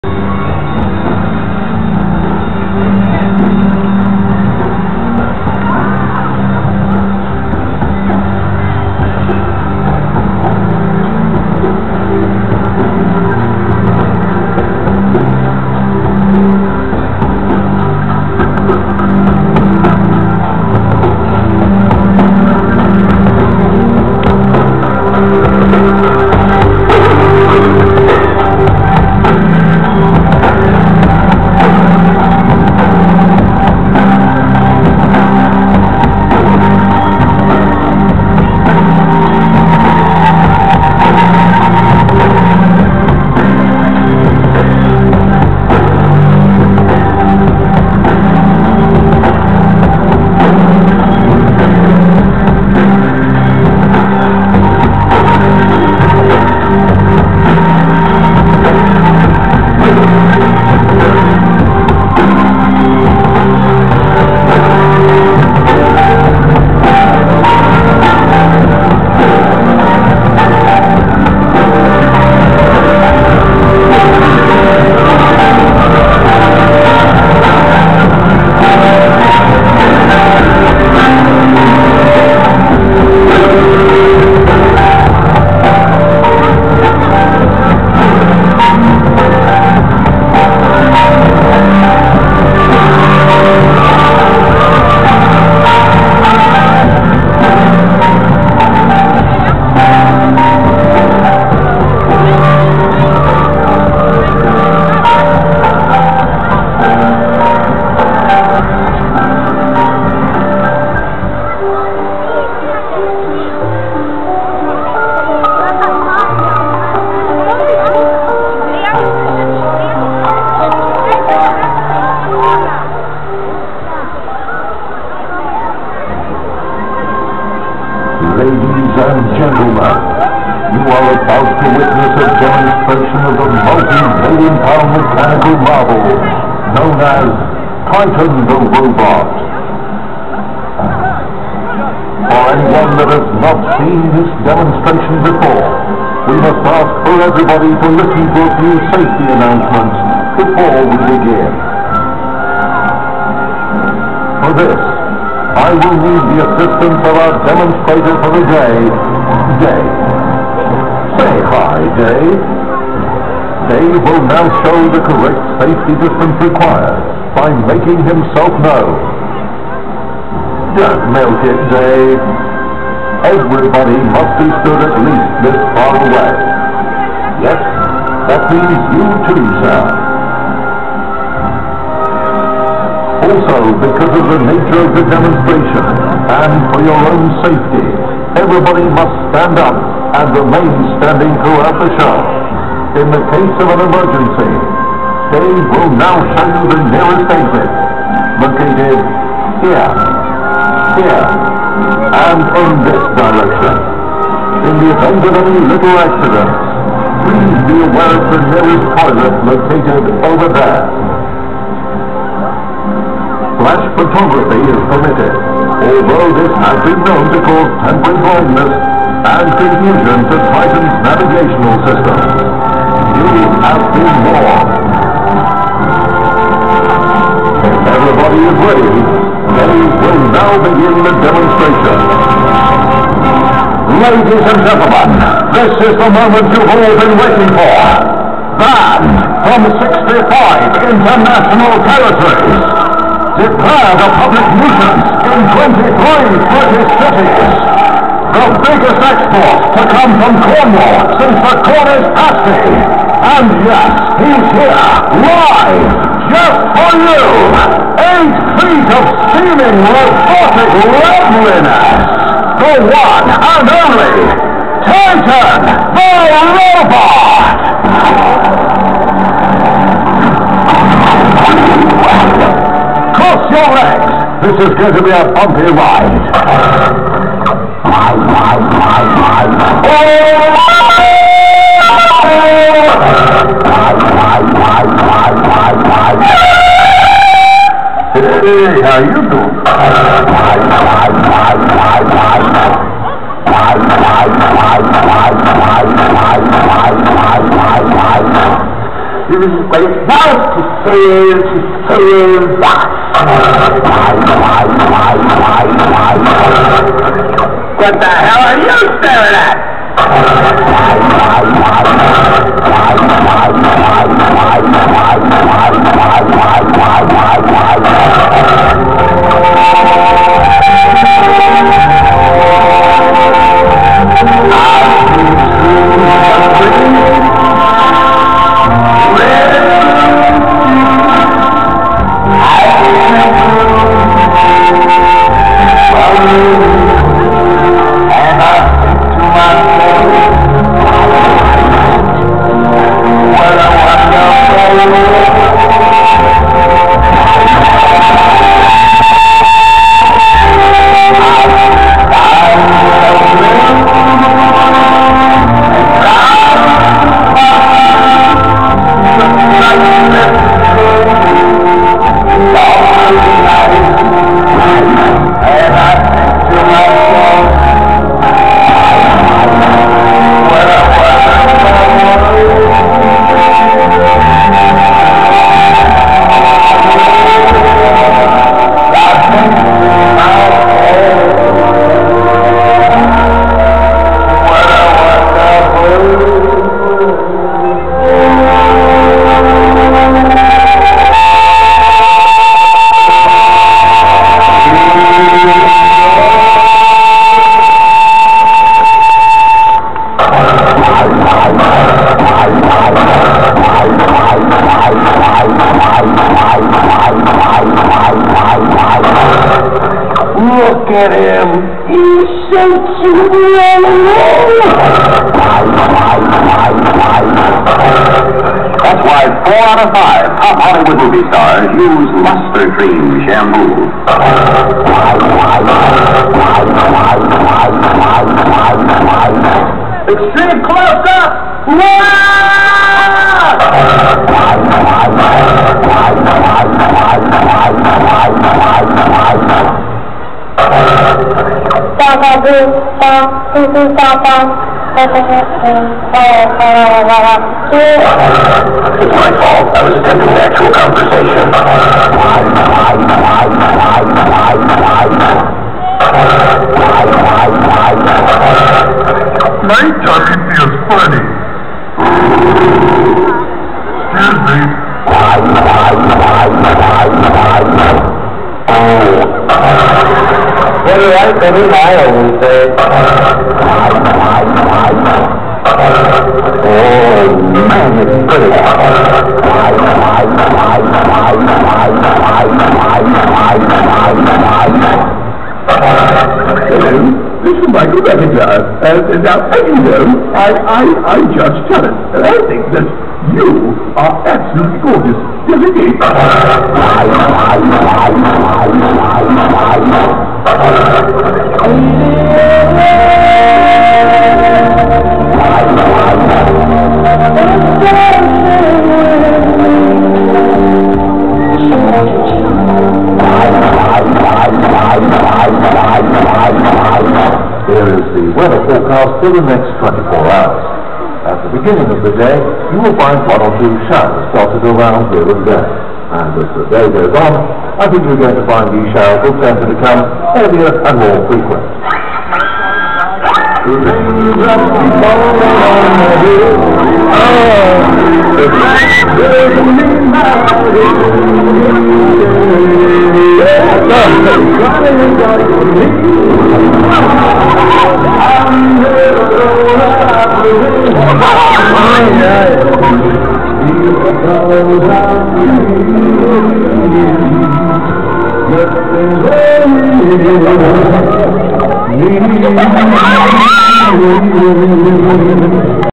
Butlins Skegness - Titan The Robot - 2011
During my visit to butlins in skegness, I was able to see, from the room where he came out, to the room where he left, TITAN. THE. ROBOT!